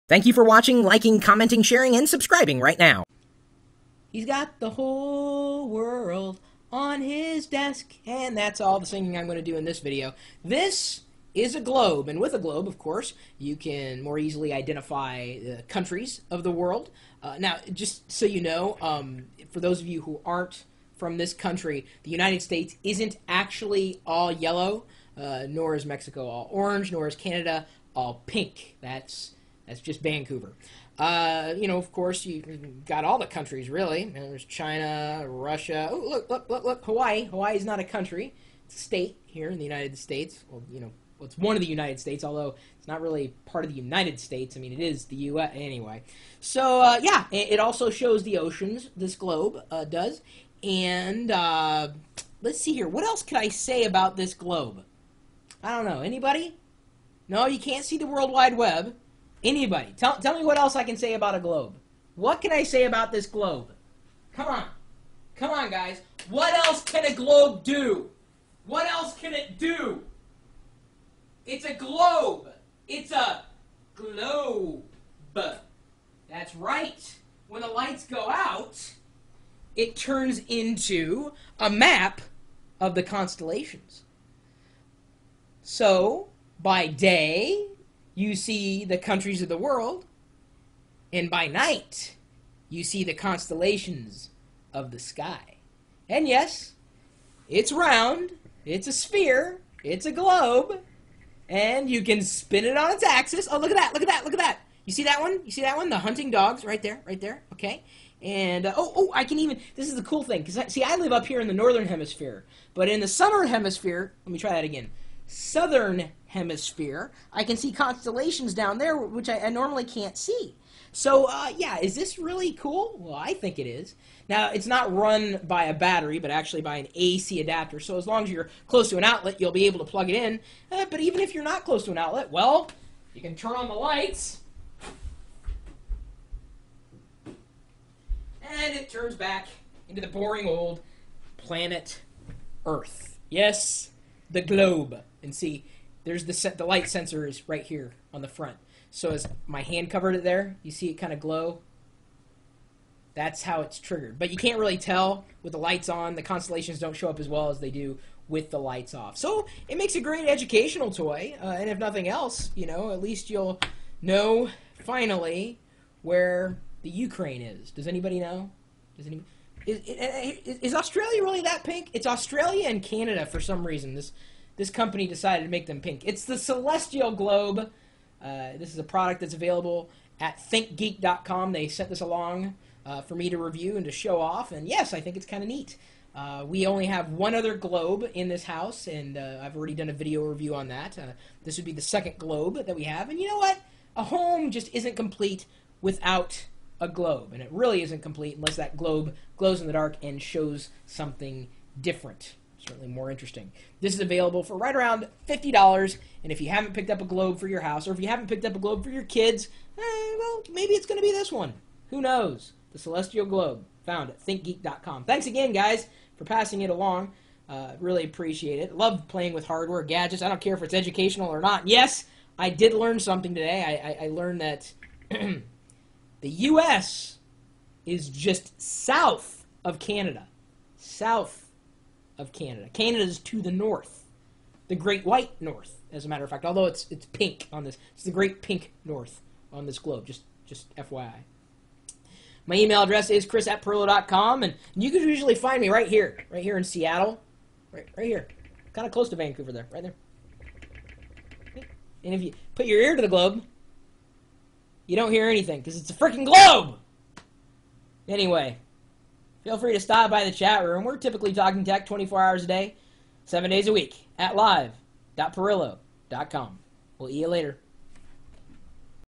Why Don't Globes Glow in the Dark?
- Tell me. What exactly can a globe do other than show you where land masses and bodies of water are located? Oh, right. It can spin around. I forgot that part. Seriously though, can't it do anything cool?!